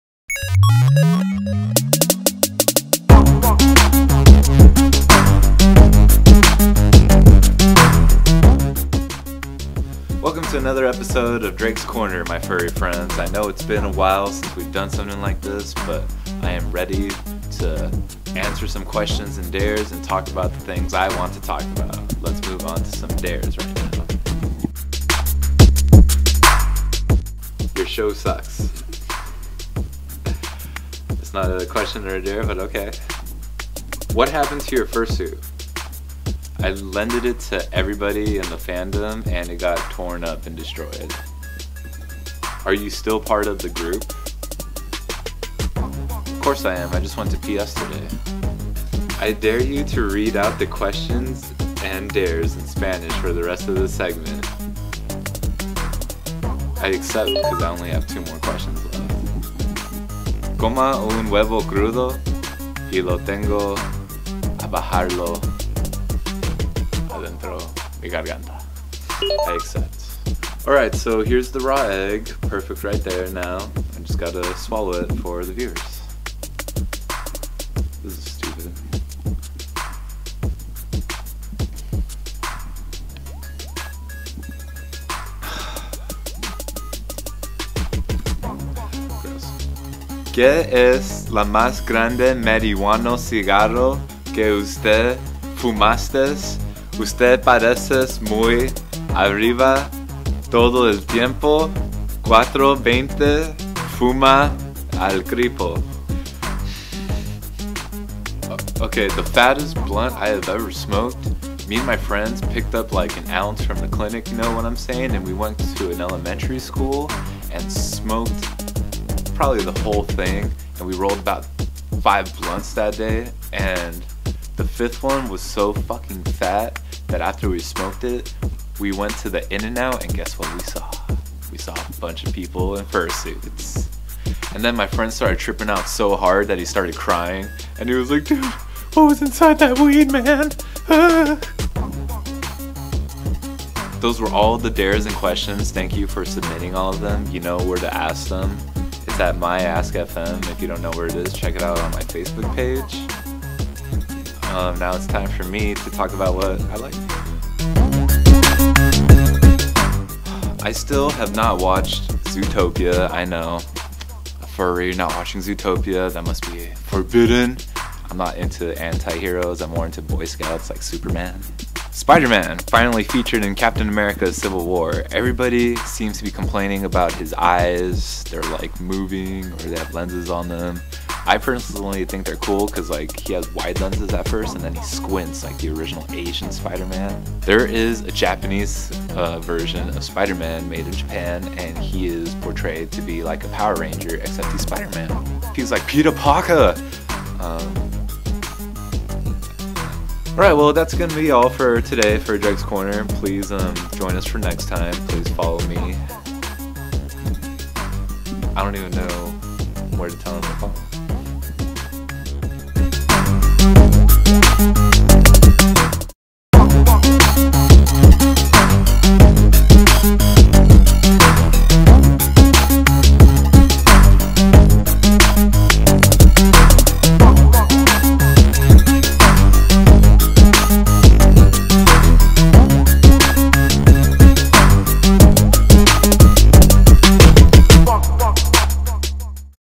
Eggs, Blunts and More! (Drake's Corner #1)
After half a year, Drake brings his segment back to the series, taking on some new questions and dares, along with a story about fat blunts and... Asian Spiderman?